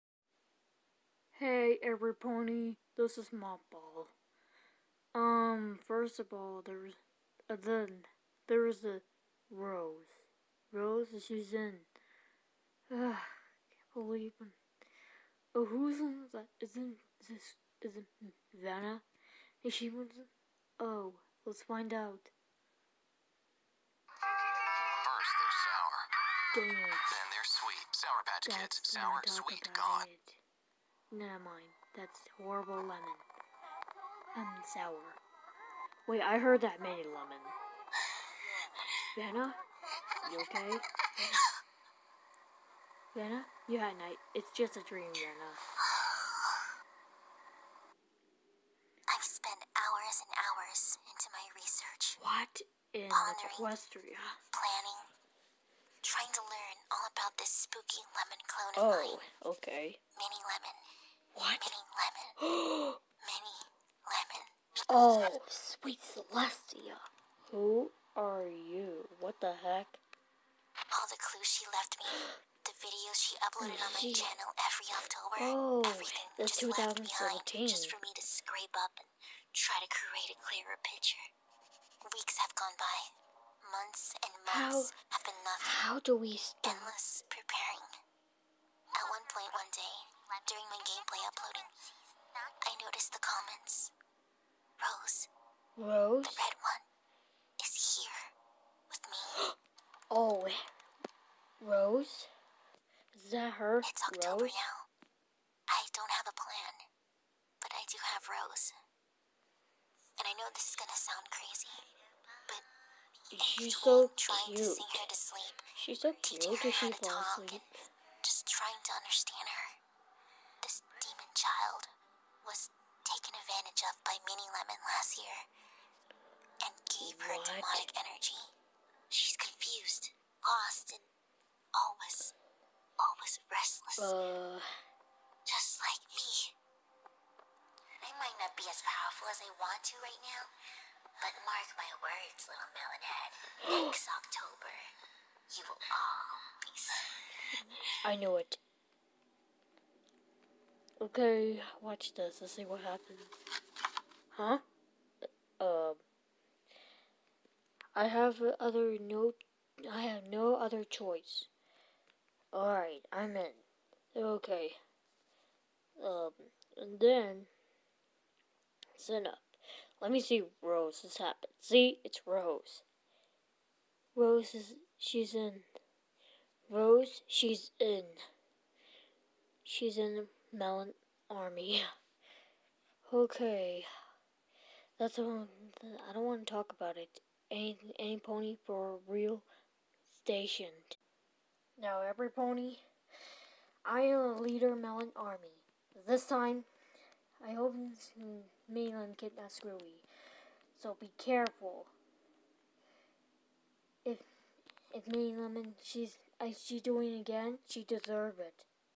Week 1 in leader of melons Army🍉🗡🍋 to:vanna🍉 and rose🍅 from:mothball I stopped mini lemon🍋
Hey Vanna and Rose I think mini lemon kidnapped screwball and that's what happens I saw this video her YouTube channel I will charge her YouTube channel I was a changeling I'm going to be leader of melons Army so Everypony and melons do not eat sour candy you had to eat sweet candy it's delicious for you good luck every pony and Melon Heads